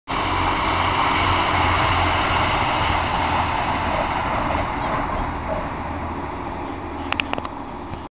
CLASS 185 - DMU - POPPLETON - 2nd JULY 2011
These sound good